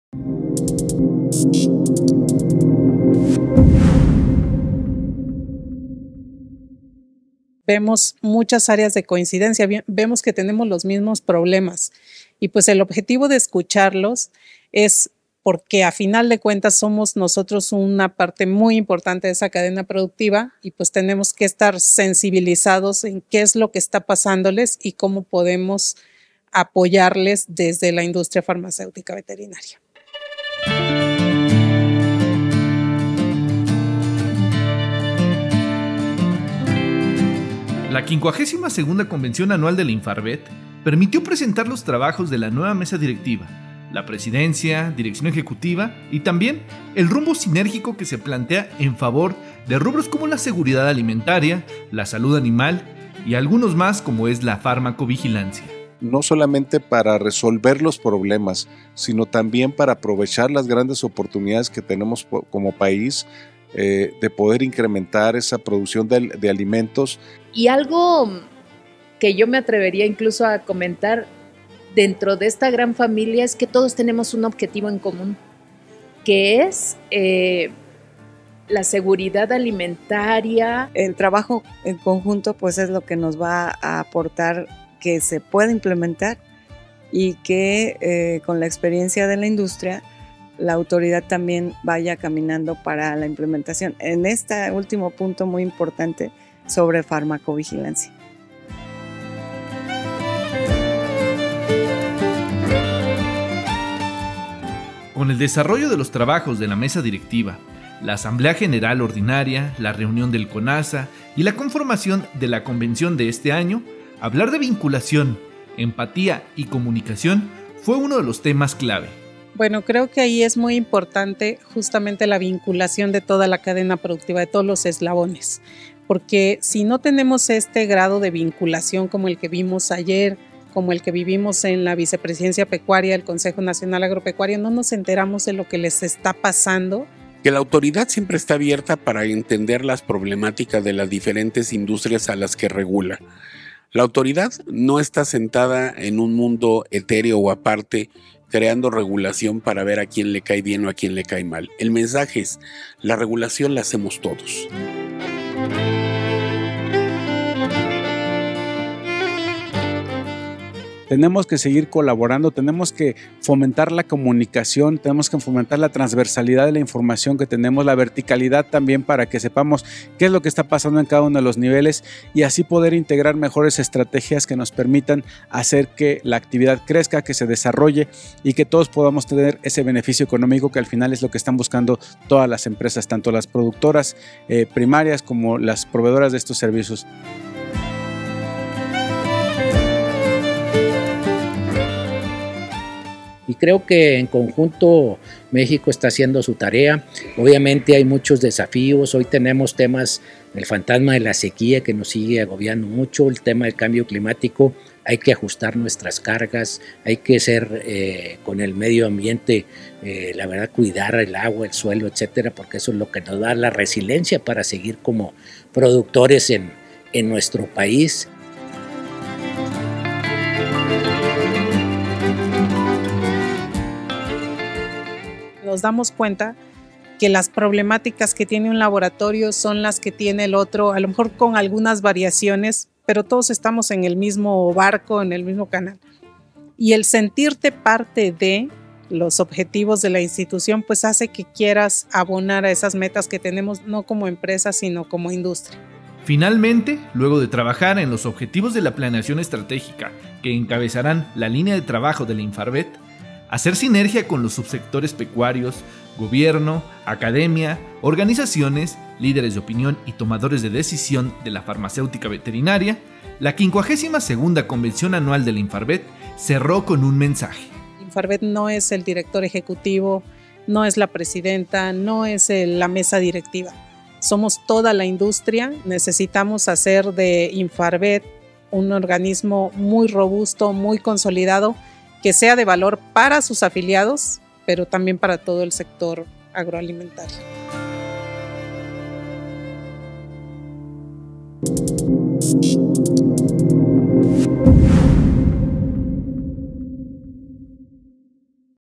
Una INFARVET robusta, consolidada, que sume valor a los afiliados y al sector agroalimentario
Este año, la Convención Anual de la INFARVET se combinó con los trabajos de la Asamblea General Ordinaria, la Reunión del Conasa y el taller de planeación estratégica… el resumen central de las actividades fue con el mensaje: “Siéntete parte de la INFARVET”.

Al asumir su papel como un aliado fundamental del sector pecuario nacional, la salud animal, los animales de compañía y la vinculación, la Industria Farmacéutica Veterinaria (INFARVET), desarrolló los trabajos de su 52º Convención Anual.

Una parte medular de este Foro, celebrado en la Ciudad de México, fue el trabajo sinérgico con el subsector productor de proteína animal, para lo cual, se invitó a participar a los voceros de los ganaderos de carne, los productores de leche, la porcicultura y la avicultura en México, el objetivo, trazar soluciones en conjunto, con efectos en el corto, mediano y largo plazo.

LA 52º CONVENCIÓN ANUAL DE LA INFARVET; UN ESPACIO QUE CONFORMÓ AL 80% DE LA FARMACÉUTICA VETERINARIA EN MÉXICO

Para ello, en estos tres días de trabajos de la Convención, también se dieron cita los representantes de los más de 25 laboratorios nacionales y multinacionales afiliados, quienes fueron testigos de la toma de protesta de la nueva Mesa Directiva del organismo para el periodo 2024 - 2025, presidida nuevamente por Gabriela Espinosa López.

Los retos, con las elecciones federales en puerta…

Durante esta jornada se brindó un espacio para que la porcicultura, la ganadería cárnica y de leche, y la avicultura presentaran un panorama del estado actual del segmento productivo, con sus logros, desafíos, áreas de oportunidad y necesidades a atender de cara a las elecciones presidenciales de este año.

A estos temas se sumaron otros de alta relevancia, tanto para la industria farmacéutica como para la producción animal, como el combate de la resistencia antimicrobiana, el entorno político y económico actual, y la problemática que representa la falsificación de medicamentos para las empresas y clientes.

El contacto con los organismos, por parte de INFARVET, propicia seguridad alimentaria

En PECUARIOS.COM platicamos con Rocío Reyes Pérez Jiménez, Directora Ejecutiva de la INFARVET, quien señaló que al estar en contacto con el segmento generador de proteína animal, pueden detectar los espacios para la vinculación y definir estrategias para hacer sinergia y hacer su parte para propiciar la seguridad alimentaria.

Luis Fernando Haro, Director general del Consejo Nacional Agropecuario (CNA), coincidió en que como industria agroalimentaria, existe la necesidad de potenciar el desempeño de cada segmento que la integra, pues solo así será posible continuar garantizando el abasto para una población global en constante crecimiento.

Cuidar la sanidad, un elemento imprescindible

Parte vital de esto es el cuidado de la sanidad, toda vez que es un elemento de la producción pecuaria imprescindible para salvaguardar el bienestar animal y propiciar un mejor rendimiento en la aportación de proteína, lo que no sería posible sin un trabajo efectivo entre todos los eslabones de la cadena y el Gobierno Federal.

Recordó que como sector, los ganaderos de leche, carne, porcicultores y avicultores, además de otras especies como ovinos y caprinos, se enfrentan a diversas amenazas en común, como la crisis climática, enfermedades, cambios en las regulaciones y la desinformación, lo que supone un llamado a abordarlos a través de la sinergia público - privada.

LA PARTICIPACIÓN DEL MÉDICO VETERINARIO EN LA SANIDAD Y LA INOCUIDAD

La Presidenta de la Federación de Colegios y Asociaciones de Médicos Veterinarios Zootecnistas de México (FedMVZ), Laura Arvizu Tovar, explicó que esta profesión, al igual que la sanidad y la inocuidad, representan uno de los principales pilares para la producción de proteína animal, representando esto un claro ejemplo de colaboración multisectorial.

Y es que al final del día, refirió, todos estos elementos, es decir, médicos veterinarios, Industria Farmacéutica, autoridades y ganaderos, forman parte de un "rompecabezas", en el que si cada pieza no cumple con su papel por medio de las vinculaciones, se vuelve más difícil alcanzar los objetivos.

La Asamblea General Ordinaria y el Taller de Planeación Estratégica, un foro para escuchar a los afiliados y definir el rumbo de trabajo

Rocío Reyes concluyó que, además de interactuar con el sector pecuario, la oportunidad de escuchar a cada uno de los laboratorios, por medio de su Asamblea General Ordinaria, sirve también para localizar las áreas de oportunidad dentro de la misma Industria Farmacéutica veterinaria nacional, en materia de regulación, etiquetados, trabajos con las autoridades de gobierno, entre otros.